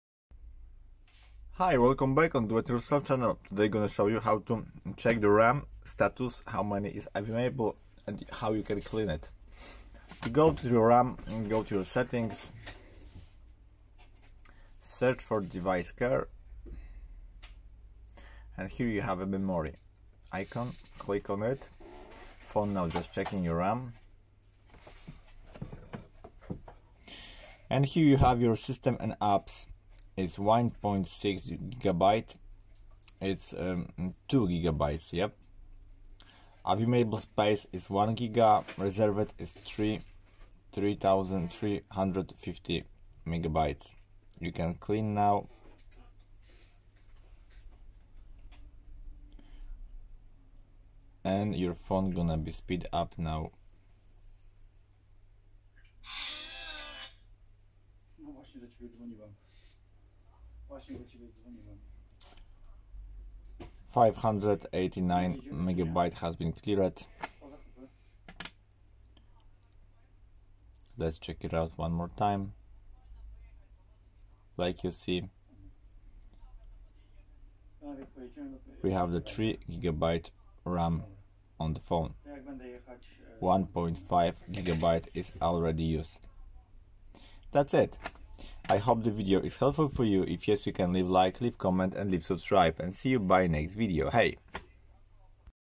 How To Check RAM Of Your Phone Samsung A20e
Hello Freinds In This Video I just show u how to check ram of your phone Remember this works for only Samsung A series